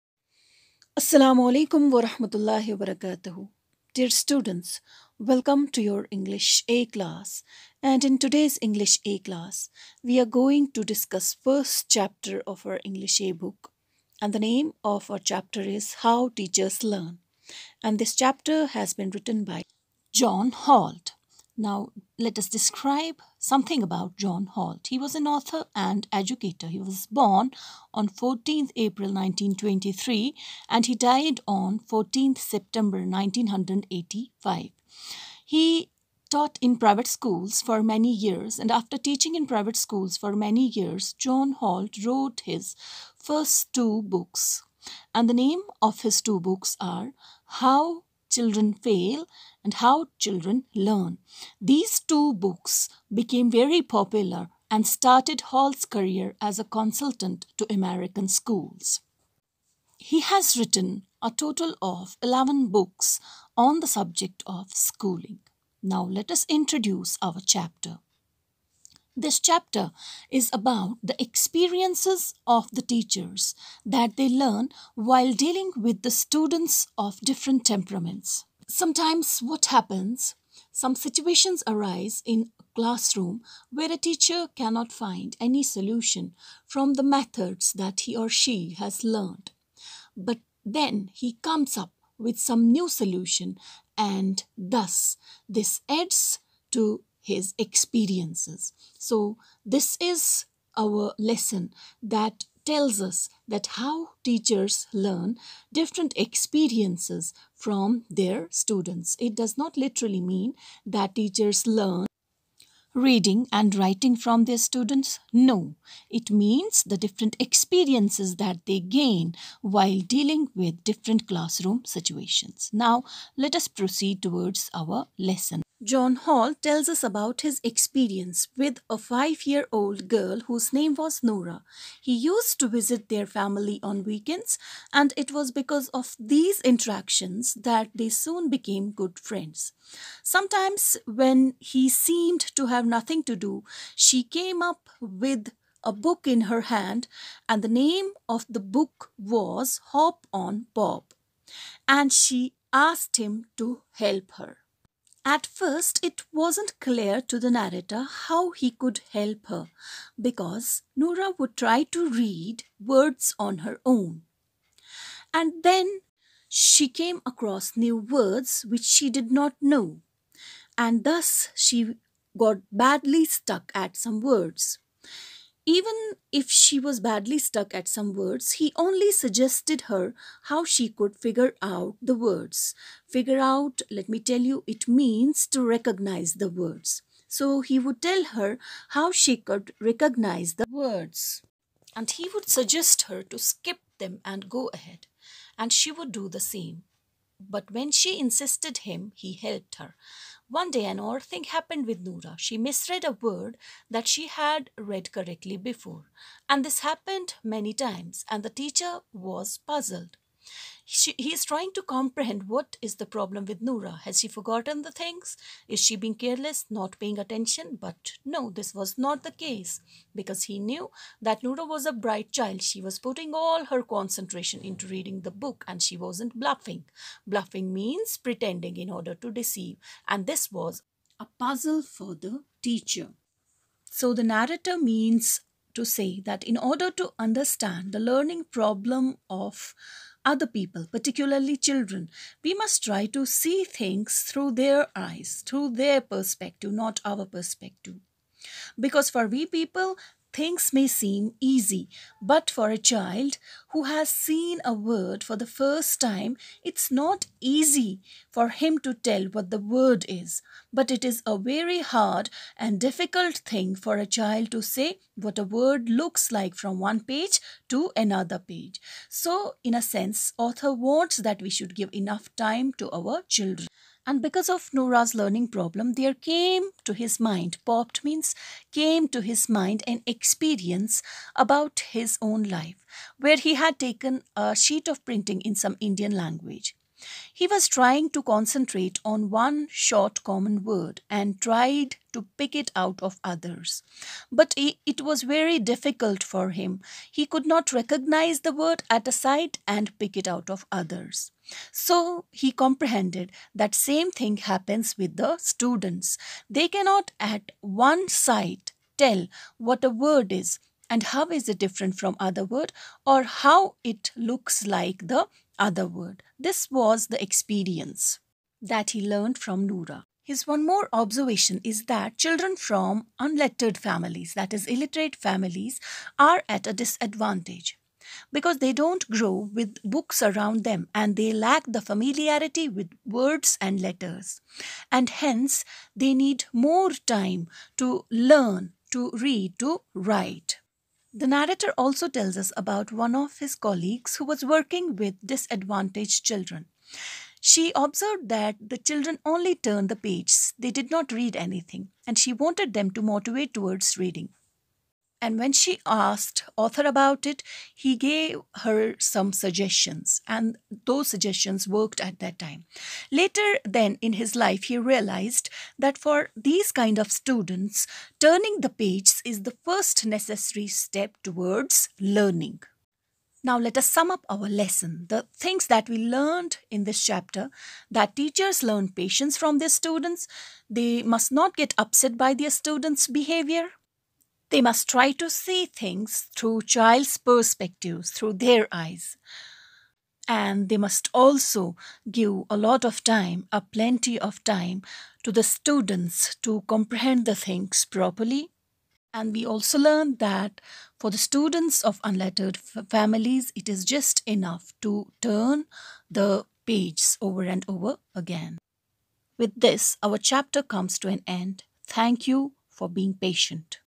Lecture #1: Class 8th English | How Teachers Learn | RP SCHOOL
Video Lecture For Class 8th
Teacher: Sameena Mam
Subject : English
Chapter 1 :  How Teachers Learn